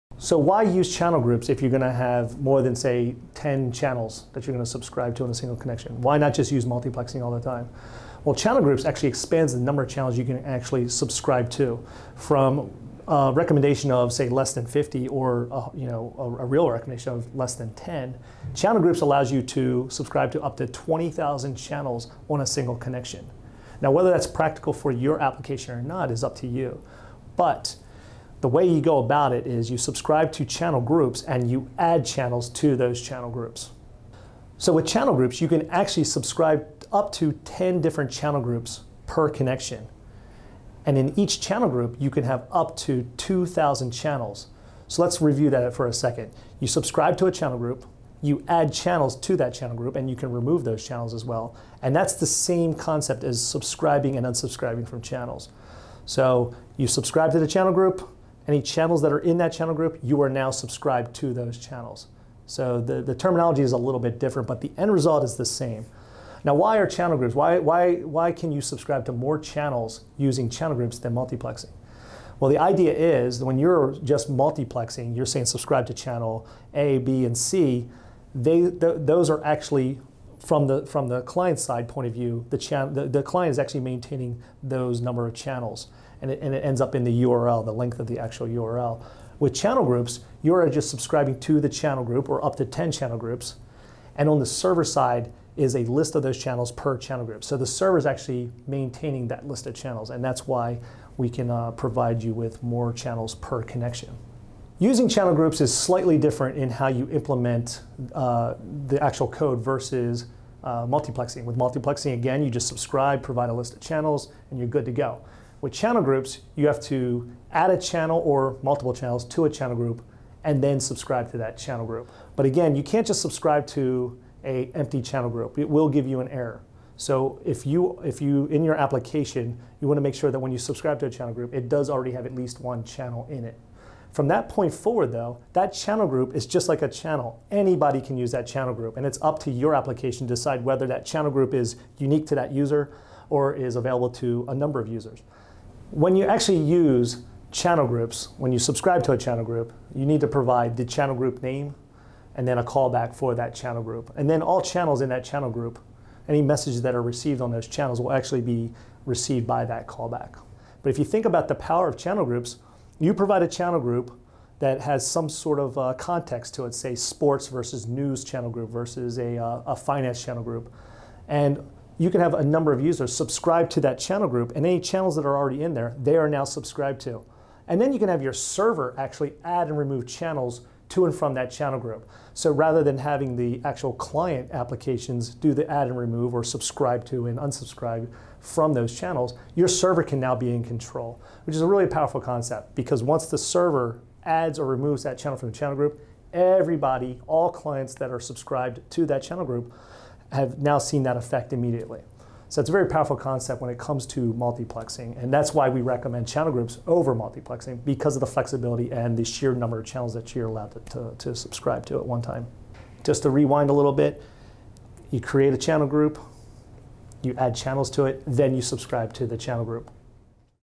UP105.4: All About Channel Groups
To work along, head over and grab your free keys from PubNub

UP105: In this course, learn about multi-channel subscription, including best practices for multiplexing and channel groups.